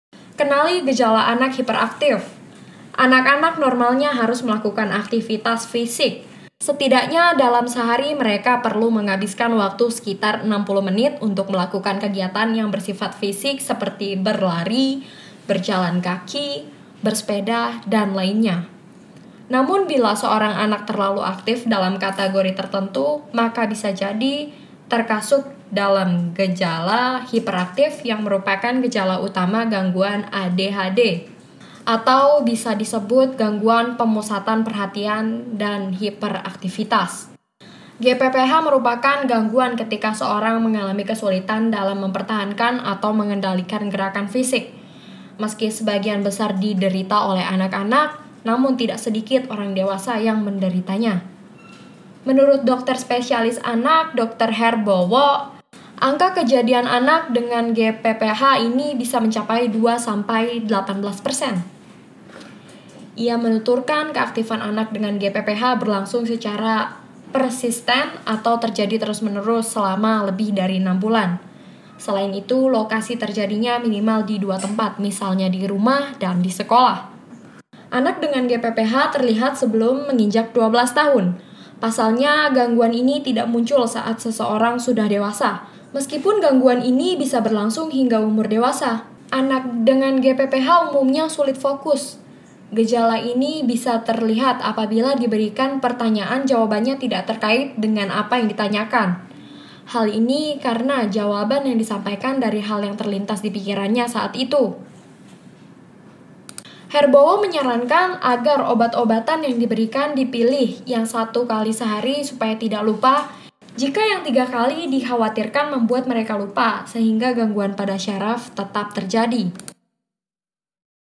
Kenali Gejala Hiperaktivitas
Anak-anak normalnya harus melakukan aktivitas fisik. Setidaknya dalam sehari mereka perlu menghabiskan waktu sekitar 60 menit untuk melakukan kegiatan yang bersifat fisik seperti berlari, berjalan kaki, bersepeda, dan lainnya. dalam gejalan hiperaktif yang merupakan gejala utama gangguan ADHD (attention deficit hyperactivity disorder), atau dalam bahasa Indonesia disebut gangguan pemusatan perhatian dan hiperaktivitas (GPPH). GPPH merupakan gangguan ketika seseorang mengalami kesulitan dalam mempertahankan atau mengendalikan gerakan fisik.  Anak dengan GPPH terlihat sebelum menginjak usia 12 tahun. Pasalnya gangguan ini tidak muncul saat sesorang sudah dewasa, meskipun gangguan ini bisa berlangsung hingga umur dewasa pada seseorang. Anak dengan GPPH umumnya sulit fokus (inatensi). Gejalan ini bisa terlihat apabila diberikan pertanyaan jawabannya tidak terkait dengan apa yang ditanyakan. Hal ini karena jawaban yang disampikan dari hal yang terlintas di pikiranya saat itu.